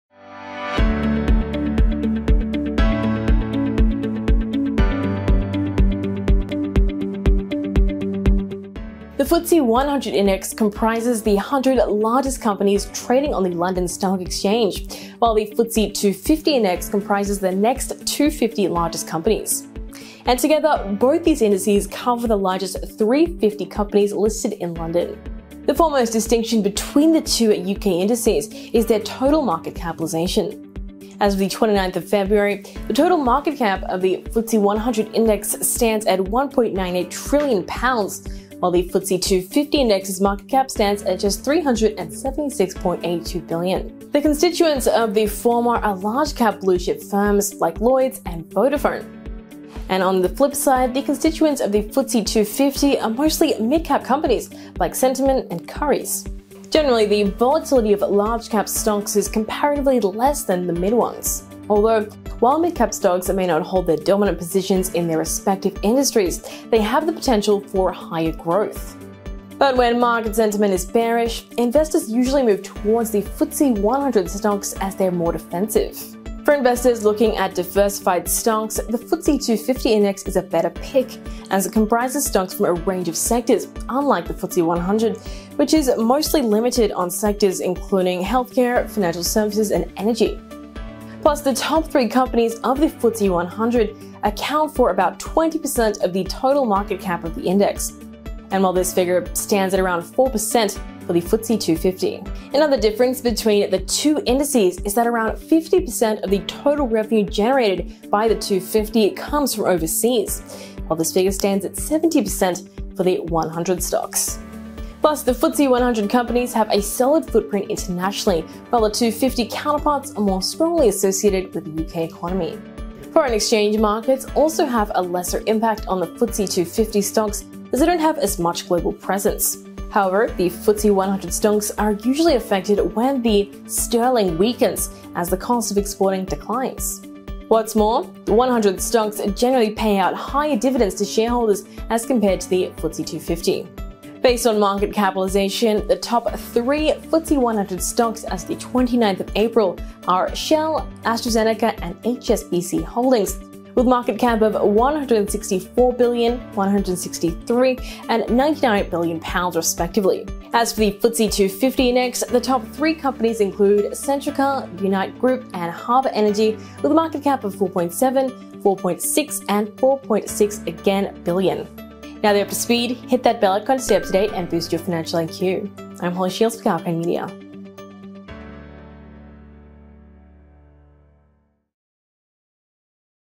What's the difference between FTSE 100 and FTSE 250?
* The FTSE 100 index comprises the 100 largest companies trading on the London Stock Exchange (LSE) while the FTSE 250 index comprises the next 250 largest companies trading on the LSE.

* The FTSE 100 companies have a solid footprint internationally, while the FTSE 250 counterparts are more strongly associated with the UK economy.

* The top three FTSE 100 stocks as of 29 April 2022 are Shell, AstraZeneca, and HSBC Holdings, with a market cap of £164 billion, £163 billion, and £99 billion, respectively while
For the FTSE 250 index, the top three companies include Centrica, Unite Group, and Harbour Energy, with a market cap of £4.7billion, £4.6 billion, and £4.6 billion, respectively.
 Understand the difference between both the indices in this video by Kalkine Media.